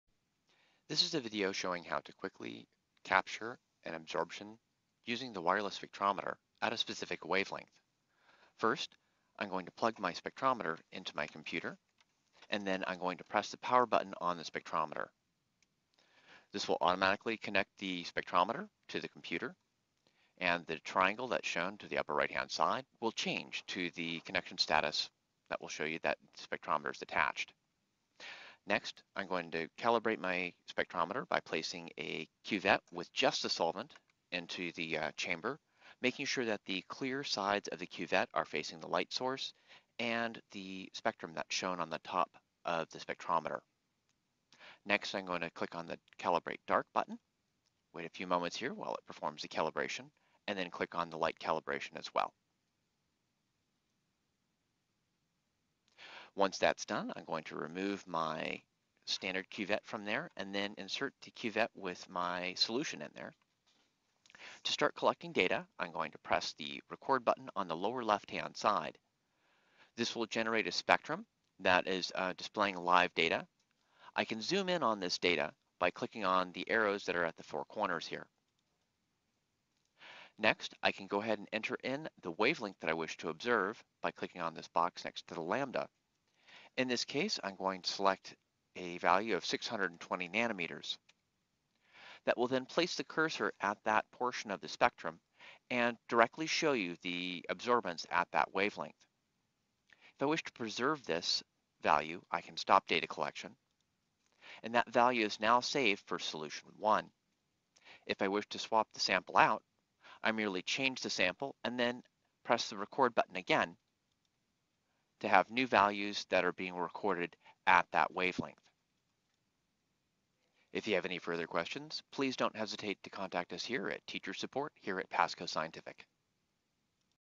Absorption Readings from Spectrometer (Spectrometry)
How do I quickly get absorbance at a prescribed wavelength from the PS-2600 Spectrometer?  To lock in the wavelength once it is selected,  click on the check button next to the wavelength.

Need more help with Spectrometry?